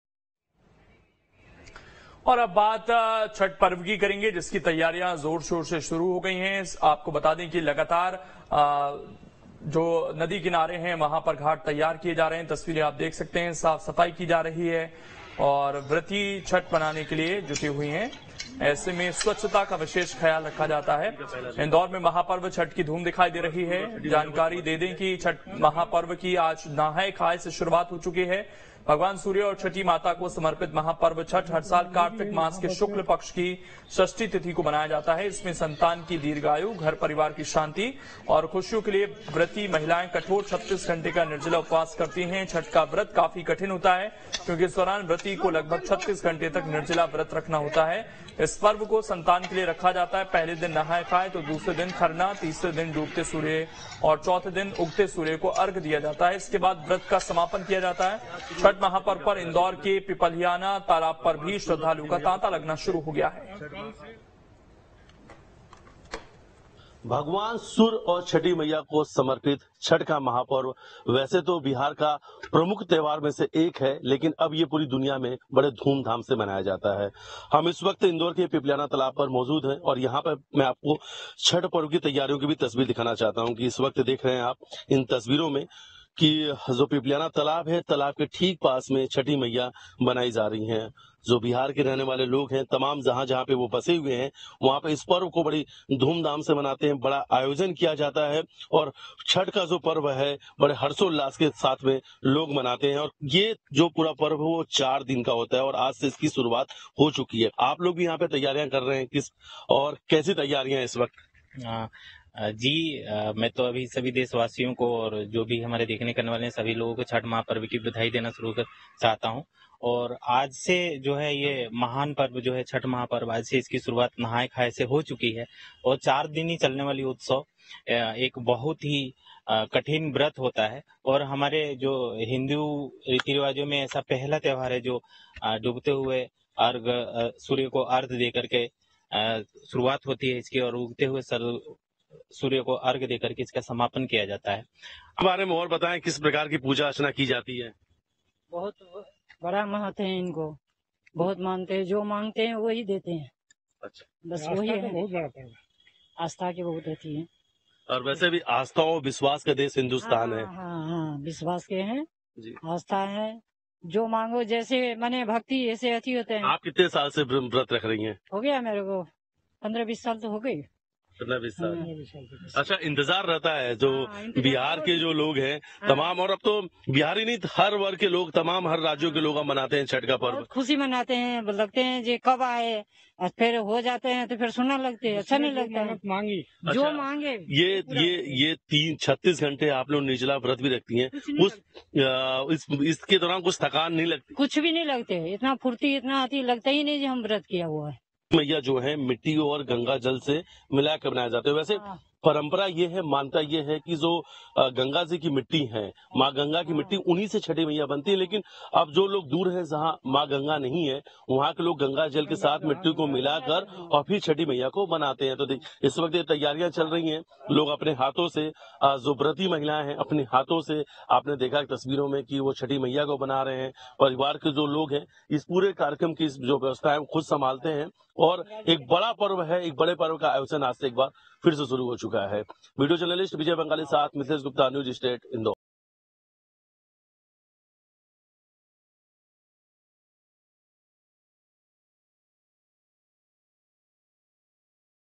Chhath Puja 2022: शुरू हुआ छठ का महापर्व, सज गए बाजार, घाटों की सफाई का काम तेज | MP News
Chhath Puja 2022: मध्य प्रदेश में छठ पूजा की तैयारी जोरो शोरों से जारी है.. घाटों की सफाई का काम तेजी से चल रहा है...

Chhath Puja 2022: शुरू हुआ छठ का महापर्व, सज गए बाजार, घाटों की सफाई का काम तेज